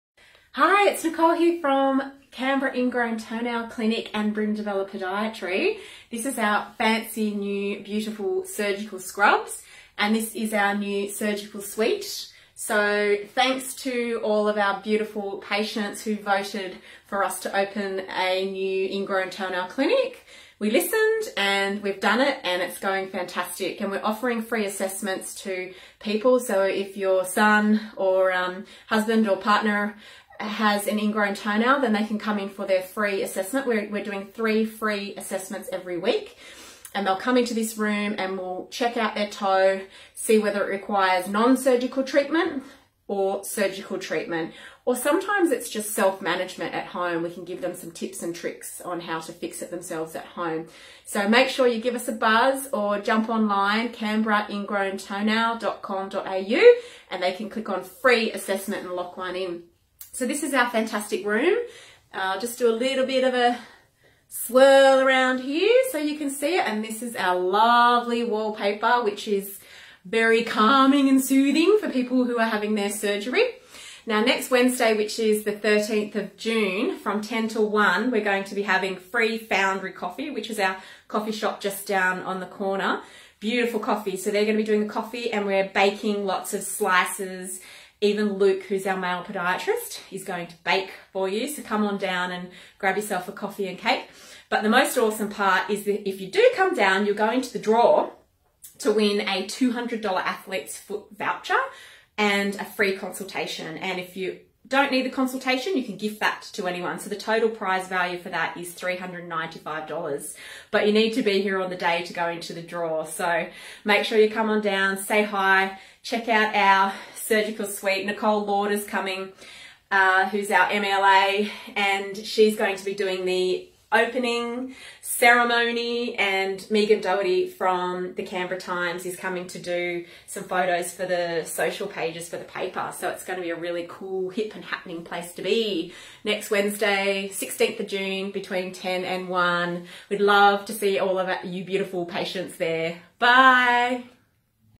CITC Open Day Video
This is our new SURGICAL SUITE called CANBERRA INGROWN TOENAIL CLINIC! Come to our OPEN DAY next week!